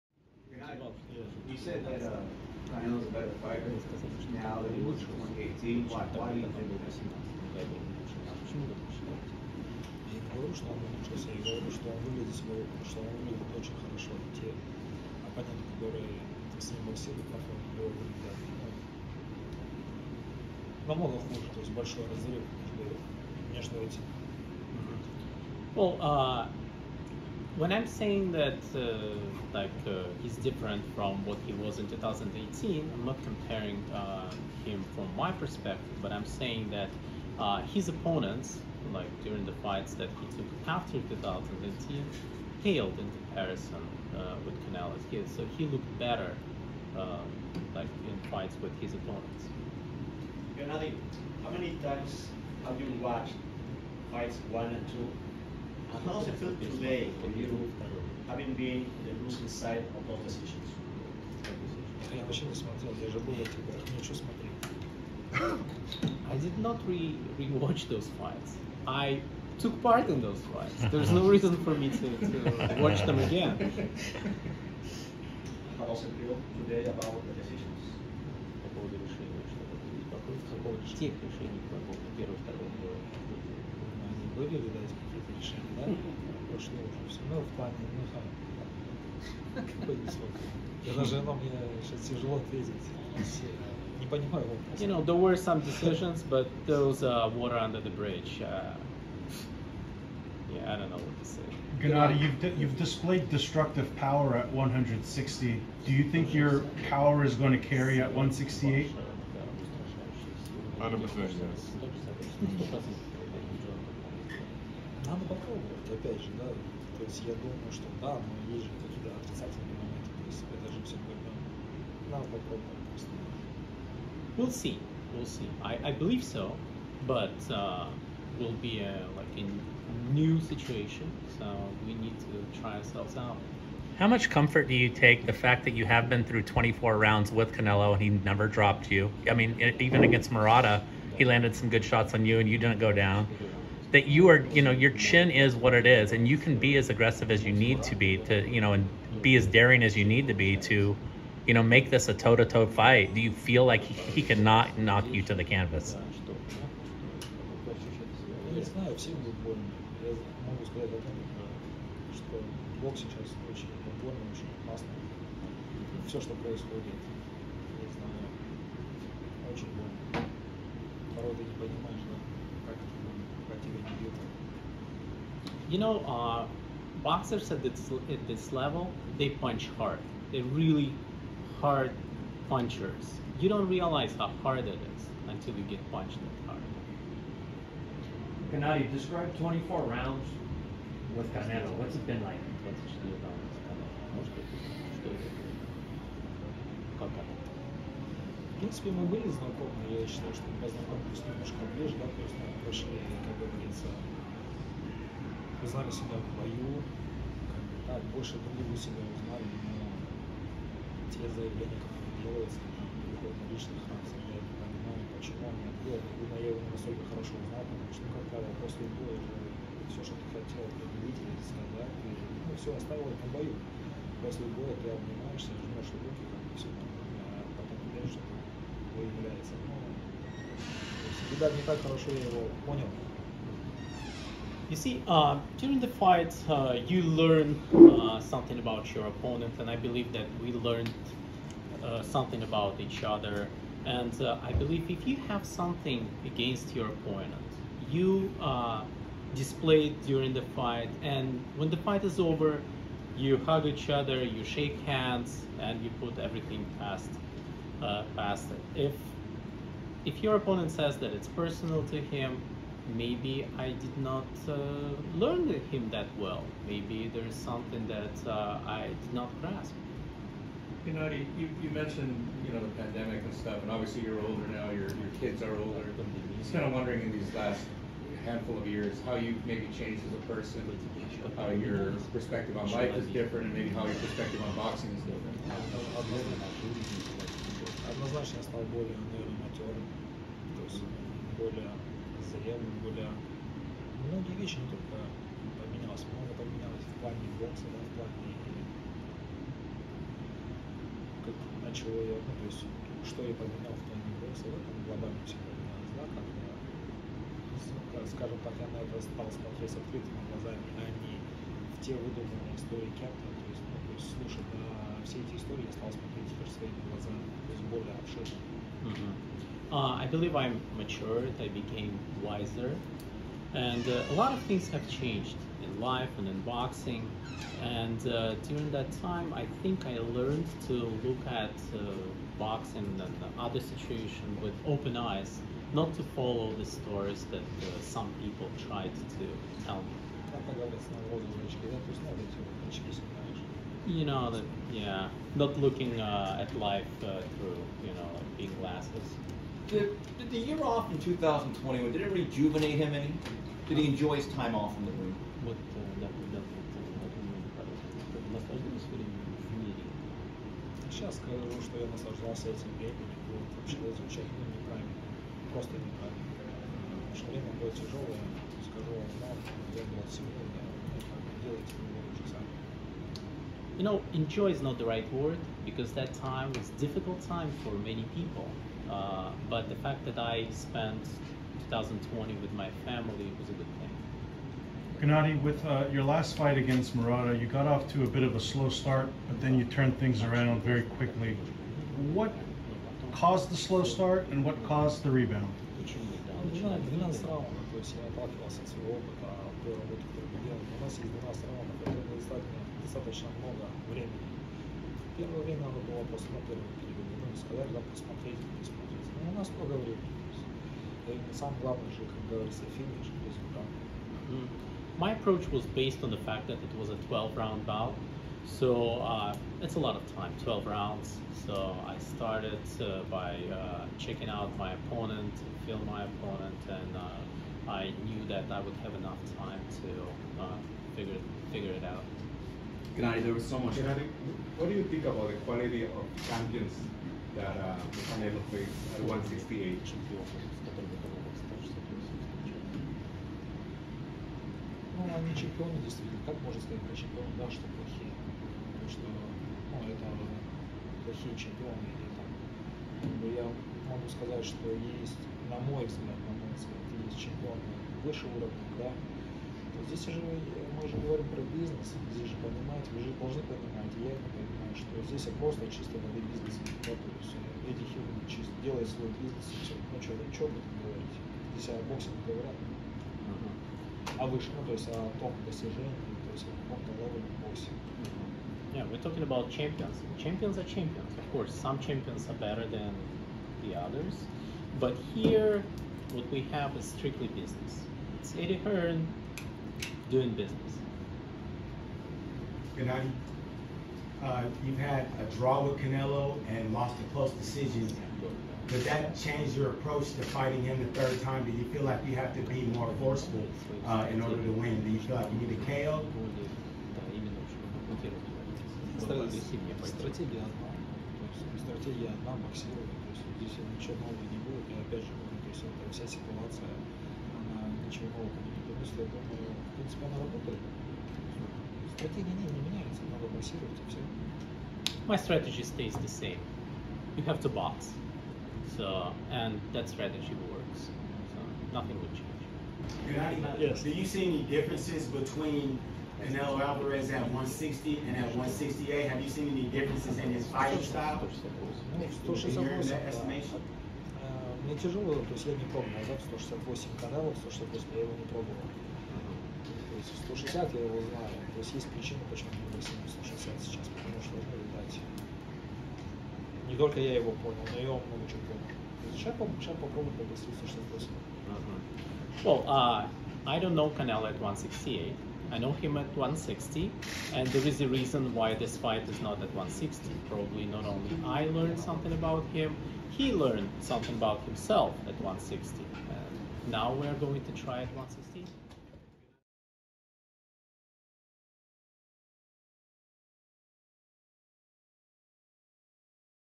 GOLOVKIN DESCRIBES FIGHTING CANELO 24 ROUNDS; AS REAL AS IT GETS ON WHAT HE LEARNED & CHANGED
FightHype.com was on the scene in Los Angeles, California where multi-division world champion Canelo Alvarez and middleweight champion Gennadiy Golovkin hosted the kickoff press conference to officially announce their upcoming September 17 trilogy showdown at T-Mobile Arena in Las Vegas, Nevada. You don't want to miss what the had to say about the fight and much more. Check it out!